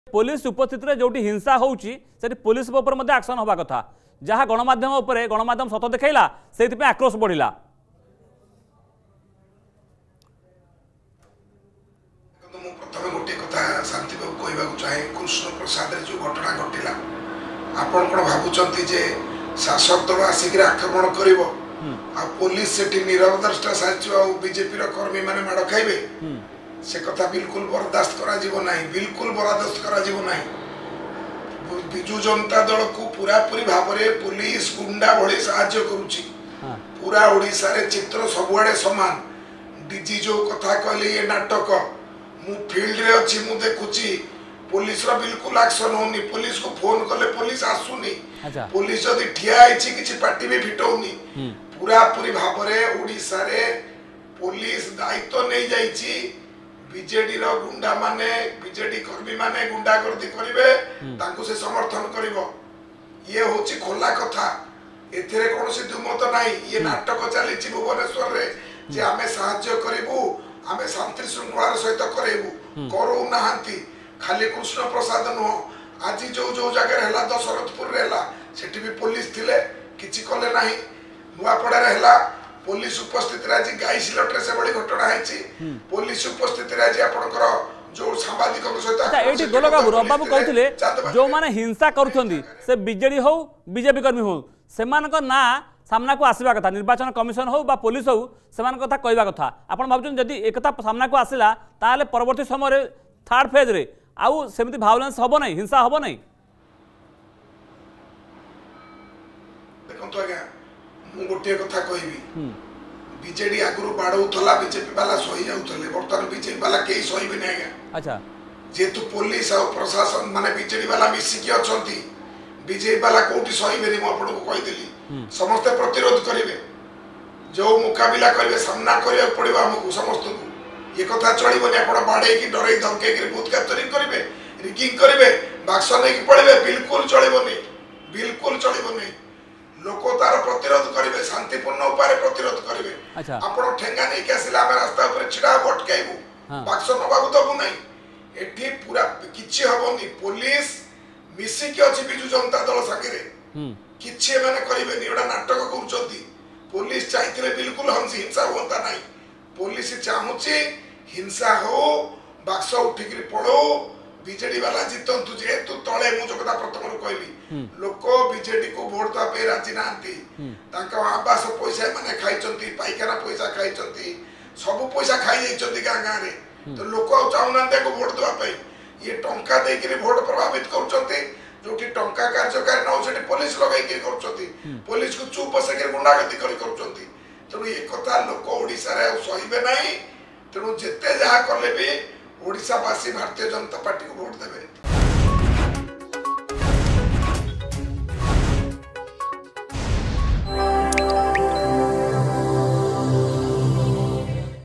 Odisha: BJP Leader Golak Mohapatra Slams BJD for Rising Violence against Opposition Supporter
ସମ୍ଵିଧାନରେ ଅହିଂସା ଗଣତନ୍ତ୍ରରେ ହିଂସା
ଶାସକ ଦଳର ଆକ୍ରମଣରେ ରକ୍ତାକ୍ତ ବିରୋଧୀ ।